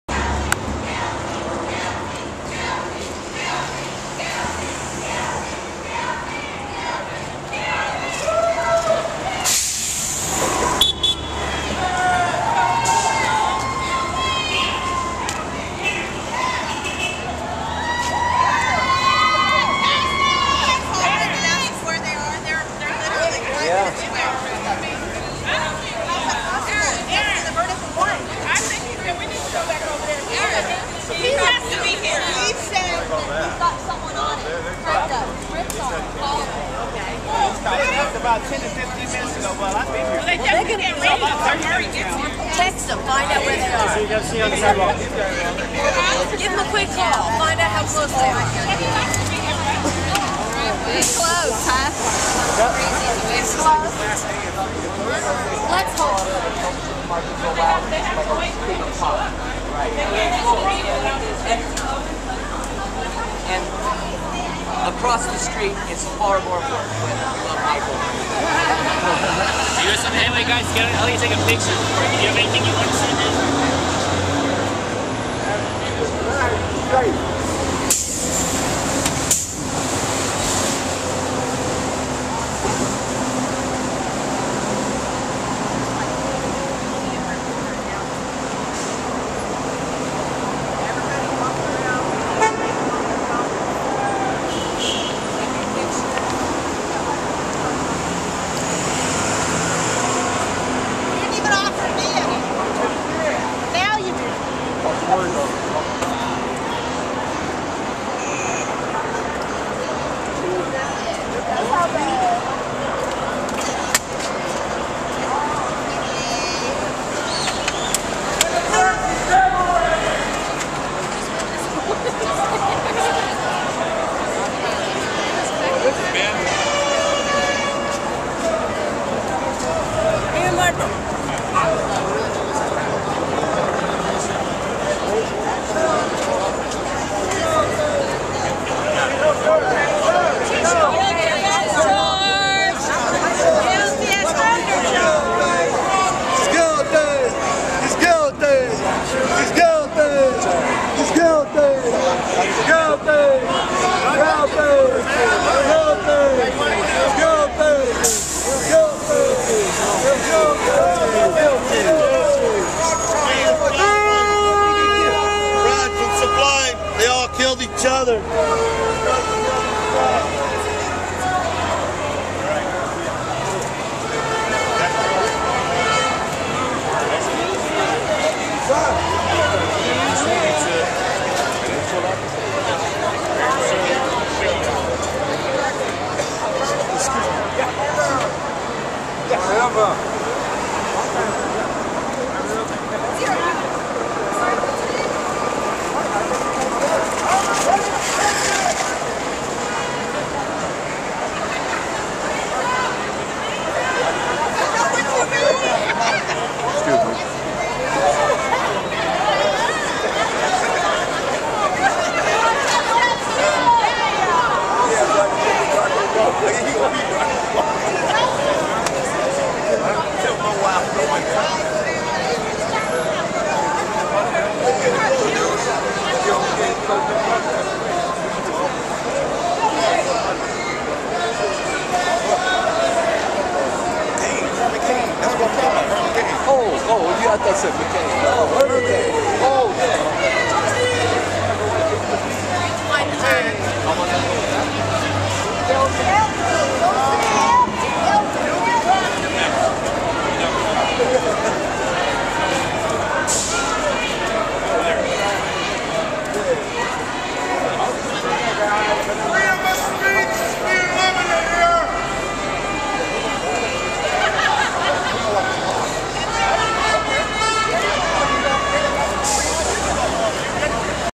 Conrad Murray Verdict in Michael Jackson Trial lady shares picture of her and Michael Jackson
An  up close and personal view of the Conrad Murray eruption on Temple st in Los Angeles